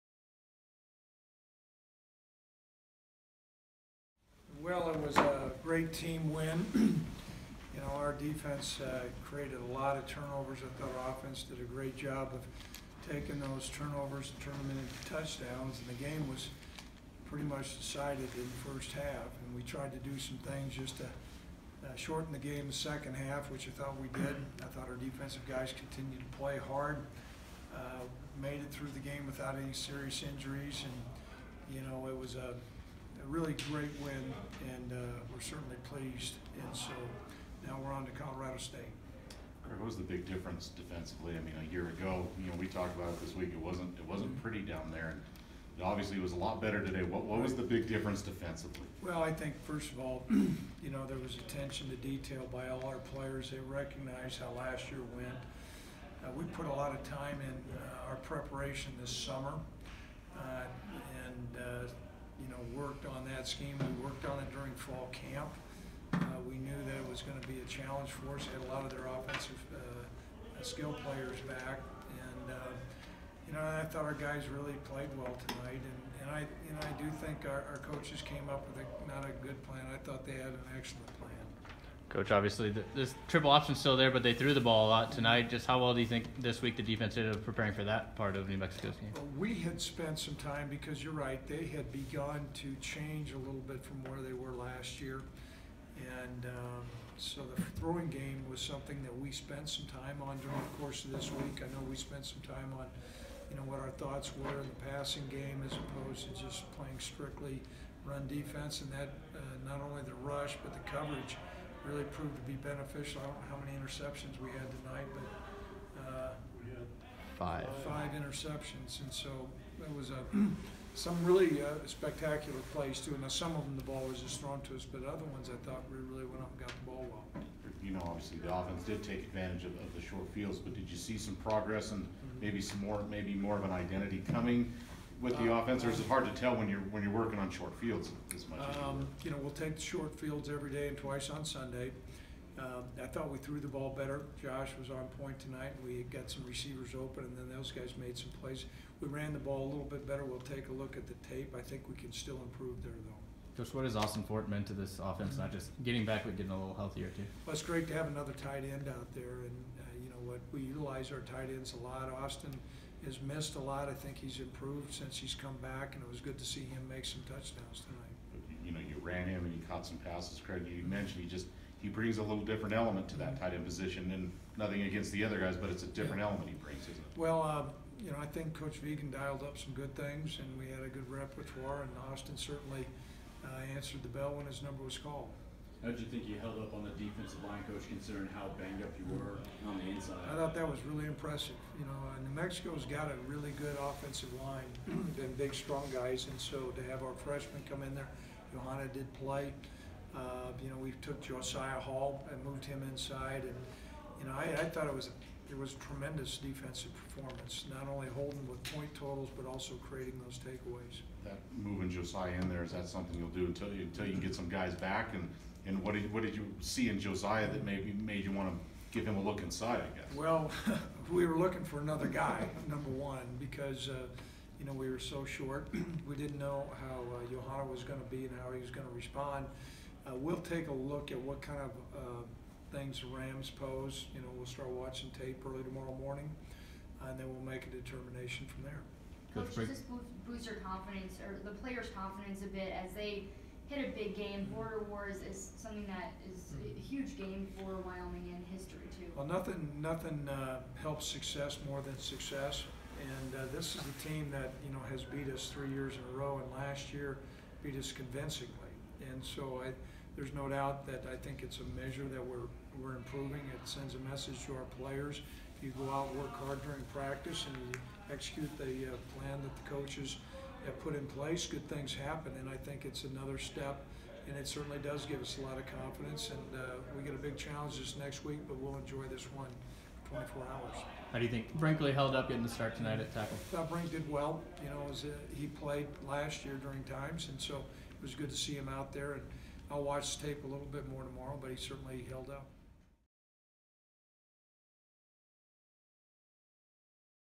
Craig Bohl discusses Wyoming's rout of New Mexico
Wyoming head coach Craig Bohl meets with media following his team's 42-3 win over New Mexico.